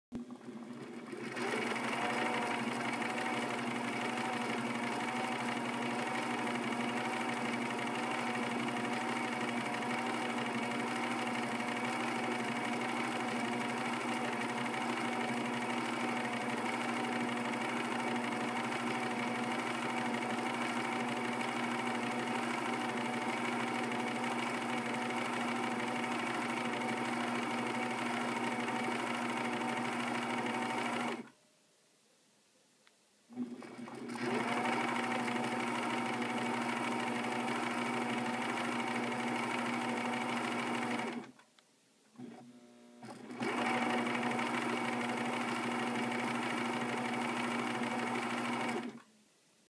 Funky-sounding motor on a Pfaff 1222e
Pfaff 1222e [P], with the foot pedal fully depressed. If you listen closely, you'll notice that, at regular intervals (about every 3 seconds), the motor wanes. I just got it back from a full oil and cleaning, but, unfortunately, that didn't help with the motor funkiness. Piercing power seems unaffected. What gives?